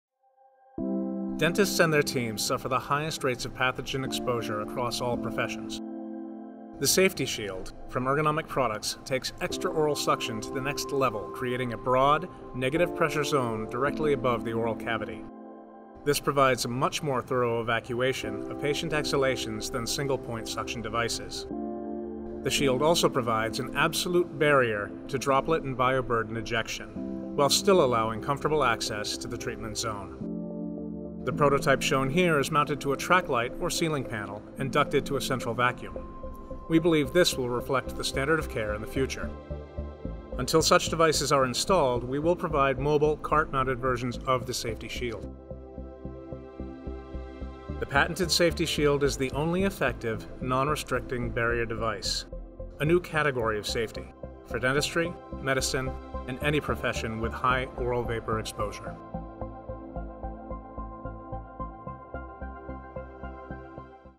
Safe-T-Shield - for any profession with a high exposure to oral vapor and aerosol
The Safe-T-Shield is a new category of safety for dentistry, medicine or any profession with a high exposure to oral vapor and aerosols.

By creating a broad negative pressure zone directly above the oral cavity, the Safe-T-Shield provides more powerful evacuation of patient exhalation than single point oral suction devices. The Shield also provides an absolute barrier to droplet and bioburden ejection while still allowing comfortable access to the treatment zone.

We believe a track-light or ceiling panel-mounted Safe-T-Shield will become the installed “Standard of Care” of the future. These devices will be ducted to a central vacuum, where exhaust fumes will undergo rigorous filtration and UV sterilization. We are researching the best methods to retrofit existing practices – and will recommend the devices in all our future designs. Until retrofitting a track-mounted Safe-T-Shield system is possible, we will offer a mobile, cart-based version for immediate deployment into your practice.

If you would like to learn more about the Safe-T-Shield, today!